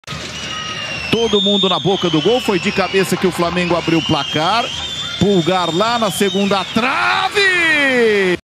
JOGADA - Pablo e Pulgar - Flamengo x Juventude [09/11/2022]
Em uma boa jogada do Pulgar o zagueiro Pablo quase marca o gol.